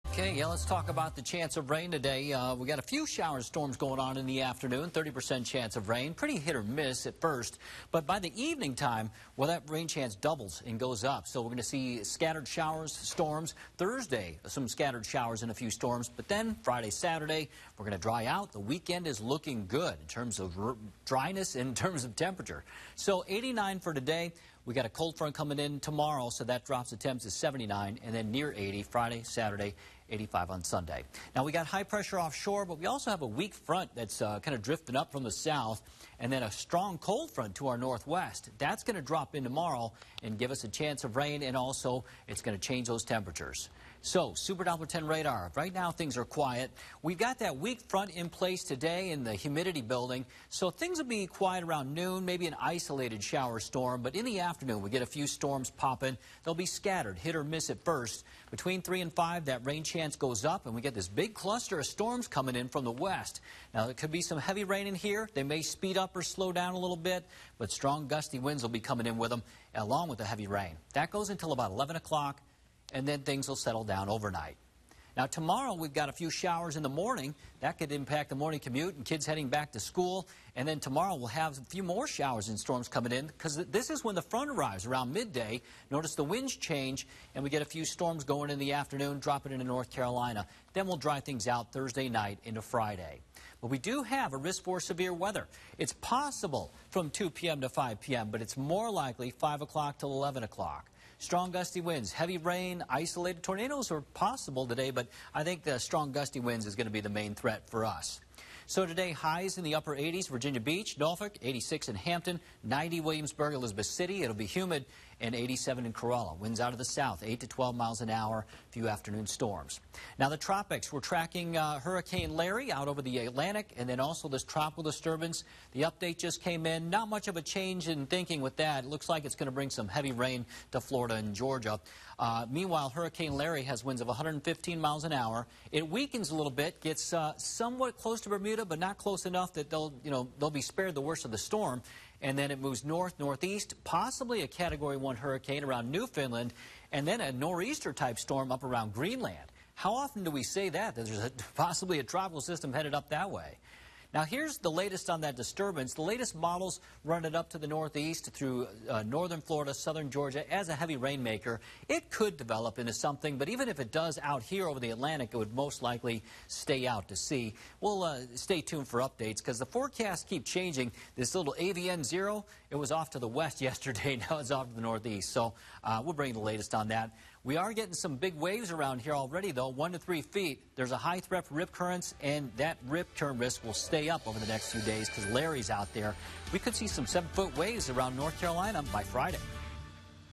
Super Doppler 10 Online Forecast | Sept. 8, 2021
Meteorologist Jeremy Wheeler with your Wednesday updated forecast.